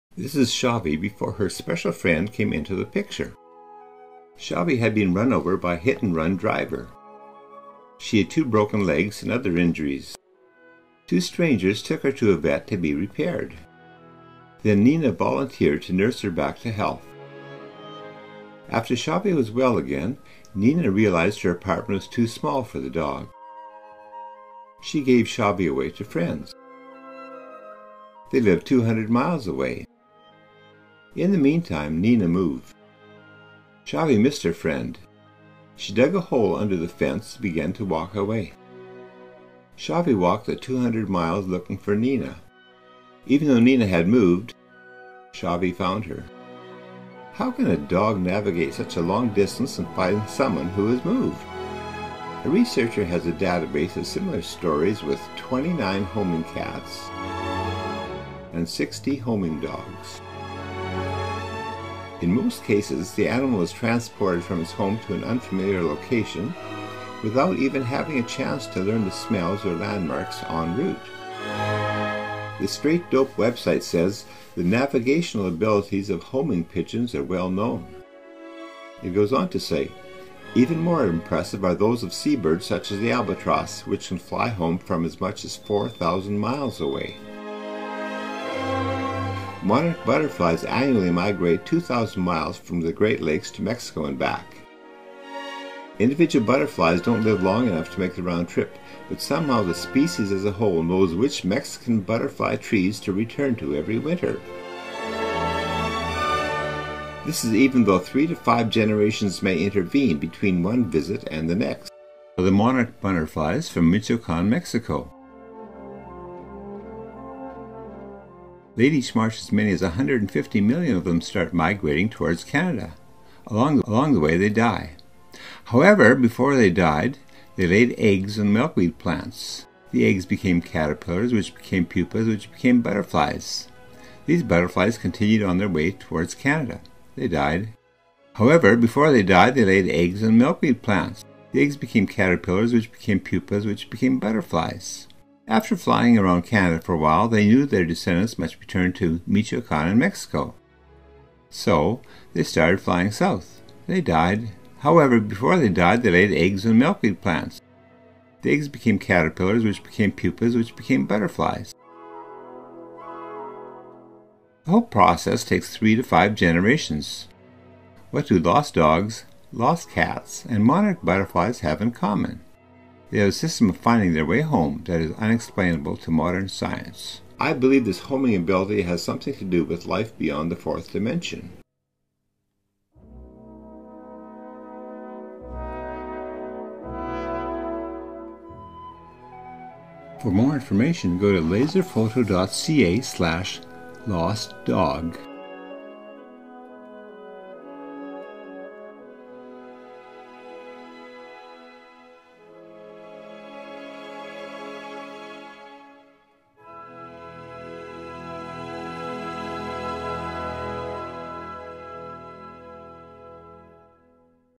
Lost Dog Walks Home -- from 200 Miles
A dog was injured by a hit and run driver.  Two strangers took Shiva the dog to a vet and then gave her to a lady named Nina.  Nina nursed Shiva back to health and then gave her to friends 200 miles away.  Shiva missed Nina so badly that she dug under the fence and walked the 200 miles to find her.  The video discusses homing behaviour of various animals including monarch butterflies.  These insects find their way from Mexico to Canada and back again even though several generation die along the way.  The great grand parents may leave Mexico and head towards Canada.  Descendants return to Mexico.  It may be the great grandchildren who knew that, when in Mexico, they must go to the state of Michoacan.